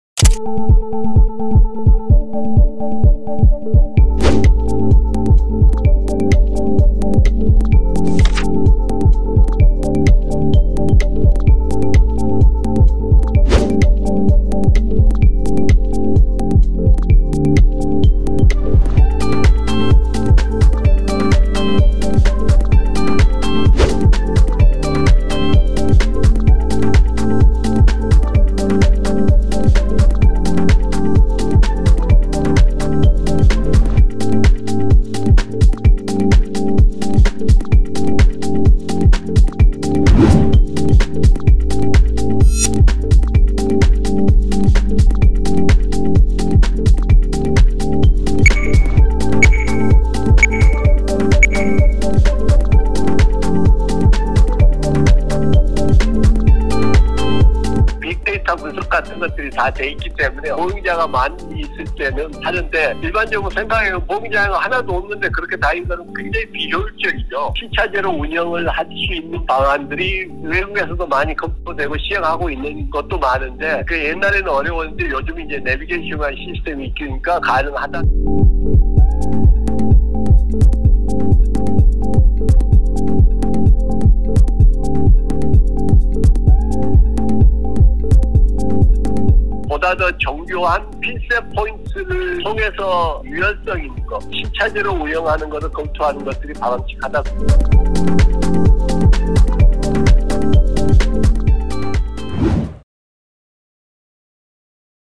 새벽 텅빈 도로도 엉금엉금하라니…무조건 천천히가 능사일까 / 연합뉴스 (Yonhapnews)
"보행자 최우선 교통환경 구축을 위해 '안전속도 5030'을 2021년 4월 17일 전면 시행하고 적극적인 홍보·계도를 거쳐 단속을 실시한다." 최근 국토교통부는 국민 생명과 안전을 지키고 교통안전 선진국으로 도약하기 위한 '2021년 교통사고 사망자 감소 대책' 세부 내용을 발표했습니다.